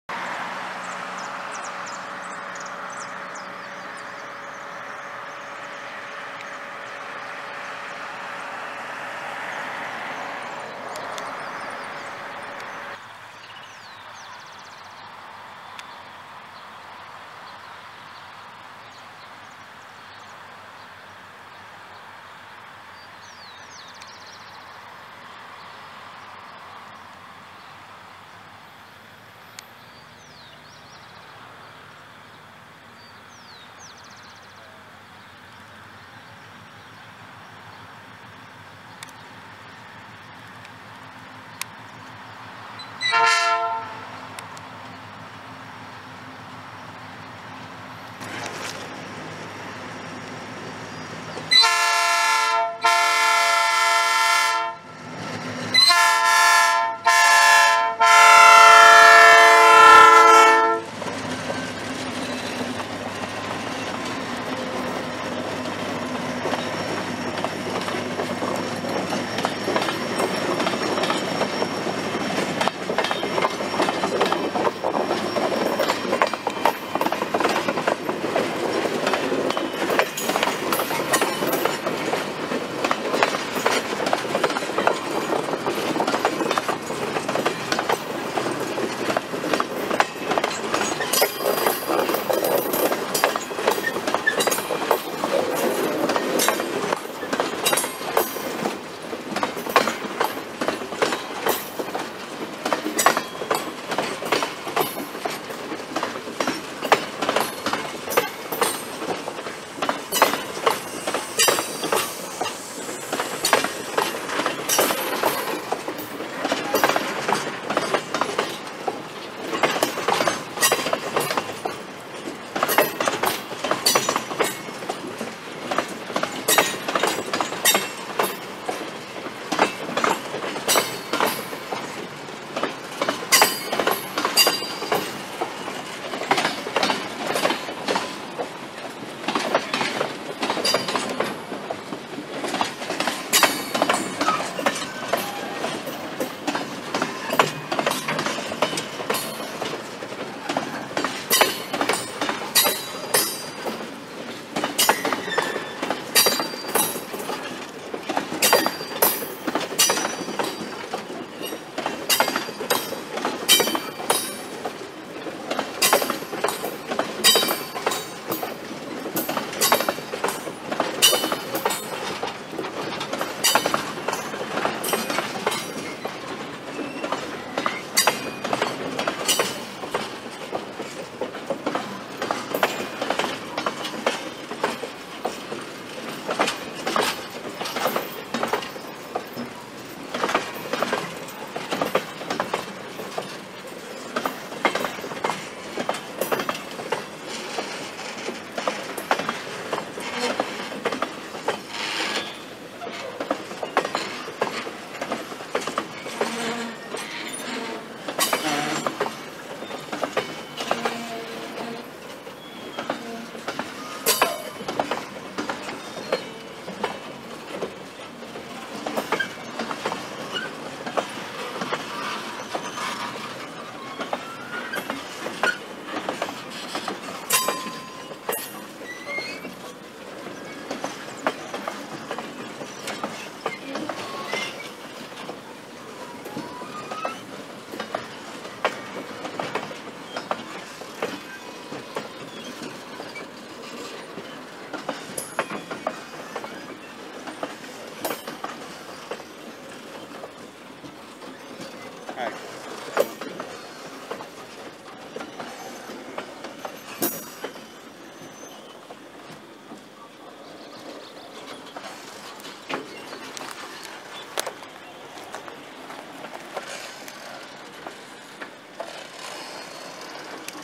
9095 N.C.A. CON CARGADO AL ATARDECER
Procedente de la Aceitera Chabás, cargado del Nuevo Central Argentino a cargo de la locomotora General Motors GT22CW 9095 doblando la curva previa a Cabin 9, para luego detenerse a esperar autorización para entrar al ramal Peyrano-Ludueña en el Km 65,8.
Pérez - provincia de Santa Fe - República Argentina - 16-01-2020

Coming from the Chabás Oil Company, loaded from the New Argentine Central to the General Motors GT22CW 9095 locomotive, bending the curve prior to Cab 9, then detect and wait for authorization to enter the Peyrano-Ludueña branch at Km 65.8.
Pérez - Santa Fe Province - Argentine Republic - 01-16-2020